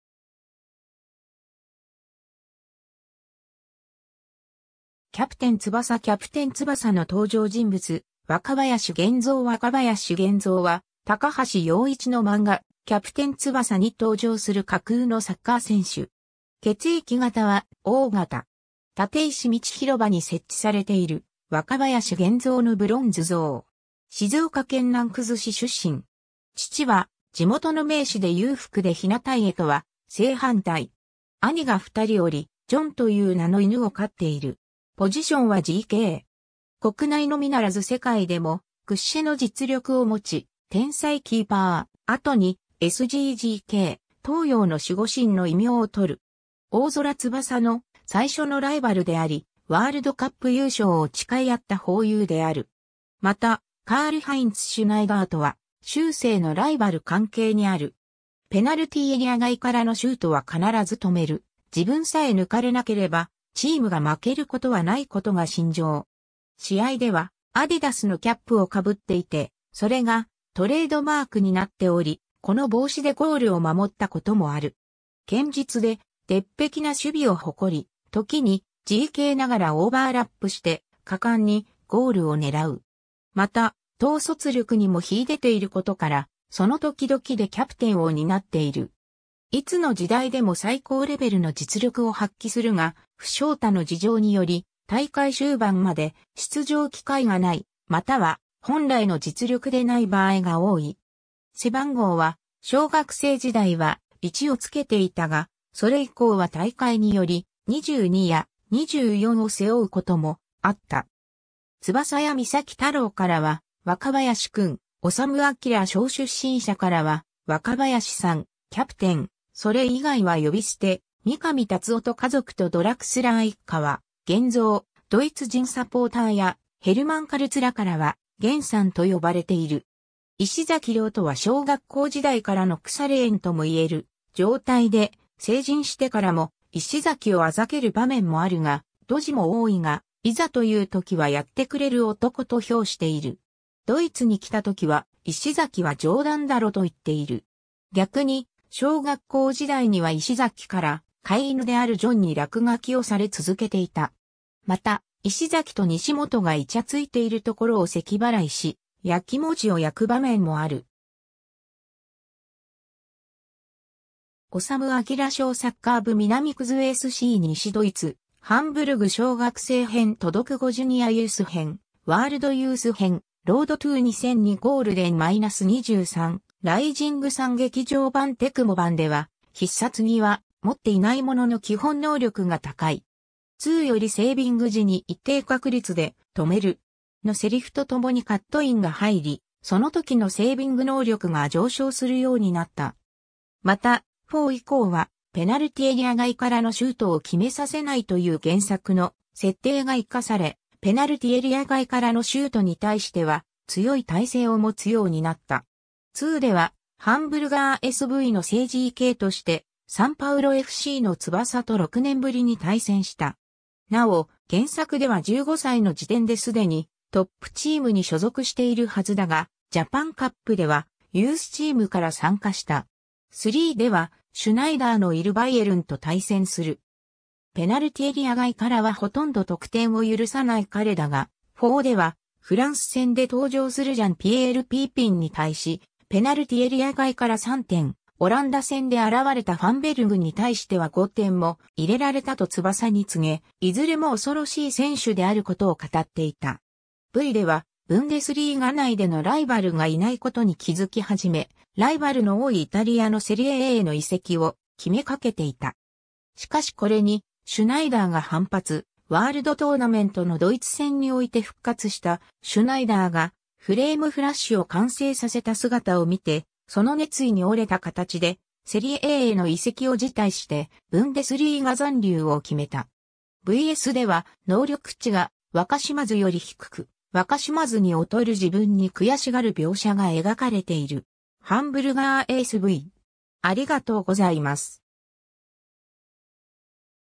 若林源三
キャプテン翼 」 キャプテン翼の登場人物 」 若林源三 若林 源三（わかばやし げんぞう）は、高橋陽一の漫画『キャプテン翼』に登場する架空のサッカー選手。
 立石みちひろばに設置されている若林源三のブロンズ像。
 静岡県南葛市出身。
父は地元の名士（大手企業グループ会長兼社長で修哲小学校や修哲中学校等を運営する「学校法人修哲学園」の理事長も勤める）で裕福で日向家とは正反対。
兄が2人(修一、栄次)おり、ジョンという名の犬を飼っている。
国内のみならず世界でも屈指の実力を持ち、「天才キーパー」、後に「S.G.G.K（スーパーグレートゴールキーパー）」「東洋の守護神」の異名をとる。
大空翼の「最初のライバルであり、ワールドカップ優勝を誓い合った朋友」である。
また、カール・ハインツ・シュナイダーとは「終生のライバル」関係にある。
 「ペナルティエリア外からのシュートは必ず止める」「自分さえ抜かれなければチームが負ける事はない」ことが信条。
試合ではアディダスのキャップを被っていて、それがトレードマークになっており、この帽子でゴールを守った事もある。
堅実で鉄壁な守備を誇り、時にGKながらオーバーラップして果敢にゴールを狙う。
また、統率力にも秀でていることから、その時々でキャプテンを担っている。
 いつの時代でも最高レベルの実力を発揮するが、負傷他の事情により大会終盤まで出場機会が無い、または本来の実力でない場合が多い。
背番号は小学生時代は「1」を着けていたが、それ以降は大会により「22」や「24」を背負う事もあった。
 翼や岬太郎からは「若林君」、修哲小出身者からは「若林さん」「キャプテン」、それ以外は呼び捨て、見上辰夫と家族とドラクスラー一家は「源三」、ドイツ人サポーターやヘルマン・カルツらからは「ゲンさん」と呼ばれている。
 石崎了とは小学校時代からの腐れ縁とも言える状態で、成人してからも石崎を嘲る場面もあるが、「ドジも多いが、いざという時はやってくれる男」と評している。
ドイツに来た時は「石崎は冗談だろ」と言っている。
逆に、小学校時代には石崎から、飼い犬であるジョンに落書きをされ続けていた。
また、石崎と西本がイチャついている所を咳払いし、焼餅を焼く場面もある。
 修哲小サッカー部（修哲小学校）- 南葛SC - 西ドイツ・ハンブルグ（平成版アニメではグリュンバルド） 小学生編 渡独後 ジュニアユース編 ワールドユース編 ROAD TO 2002 GOLDEN-23 ライジングサン 劇場版 テクモ版では必殺技は持っていないものの基本能力が高い。
「II」よりセービング時に一定確率で「とめる！」の台詞と共にカットインが入り、その時のセービング能力が上昇するようになった。
また、「IV」以降はペナルティエリア外からのシュートを決めさせないという原作の設定が活かされ、ペナルティエリア外からのシュートに対しては強い耐性を持つようになった。
 「II」ではハンブルガーSVの正GKとして、サンパウロFCの翼と6年ぶりに対戦した。
なお、原作では15歳の時点で既にトップチームに所属しているはずだが、ジャパンカップではユースチームから参加した。
 「III」ではシュナイダーのいるバイエルンと対戦する。
 ペナルティエリア外からはほとんど得点を許さない彼だが、「IV」ではフランス戦で登場するジャン・ピエール・ピピンに対しペナルティエリア外から3点、オランダ戦で現れたファンベルグに対しては5点も入れられたと翼に告げ、いずれも恐ろしい選手であることを語っていた。
 「V」ではブンデスリーガ内でのライバルがいないことに気づき始め、ライバルの多いイタリアのセリエAへの移籍を決めかけていた。
しかしこれにシュナイダーが反発、ワールドトーナメントのドイツ戦において復活したシュナイダーがフレイムフラッシュを完成させた姿を見て、その熱意に折れた形でセリエAへの移籍を辞退してブンデスリーガ残留を決めた。
 「VS」では能力値が若島津より低く、若島津に劣る自分に悔しがる描写が描かれている。
 ハンブルガーSV（II-V）